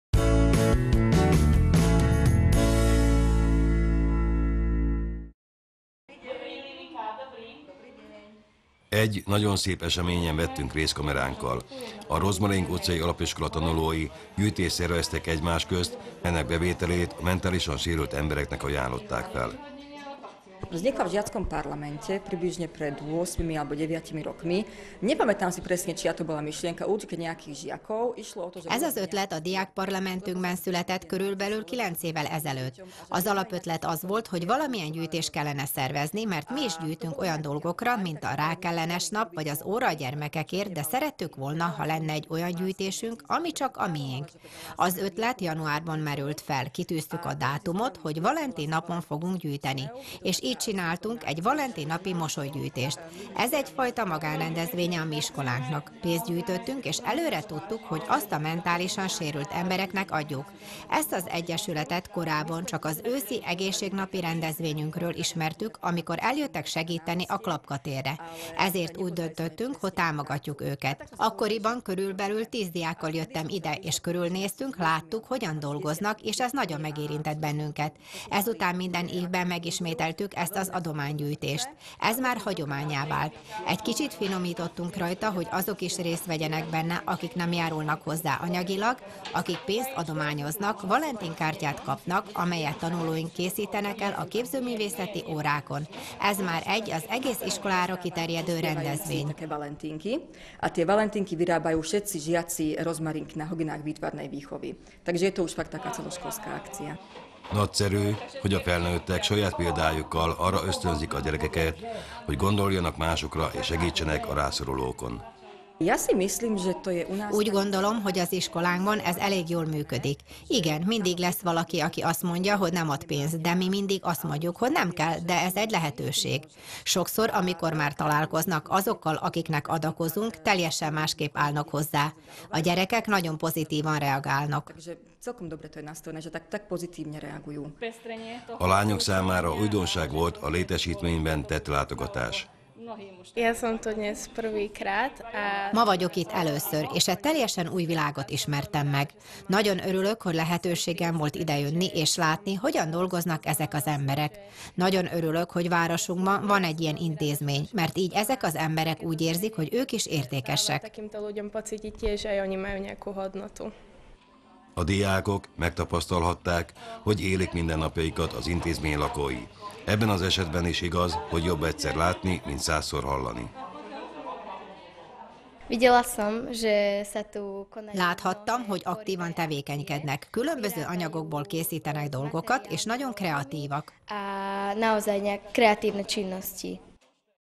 A Rozmaring Utcai Alapiskola tanulói gyűjtést szerveztek
Egy nagyon szép eseményen vettünk részt kameránkkal. A Rozmaring Utcai Alapiskola tanulói gyűjtést szerveztek egymás közt, melynek bevételét a mentálisan sérült embereknek ajánlottak fel.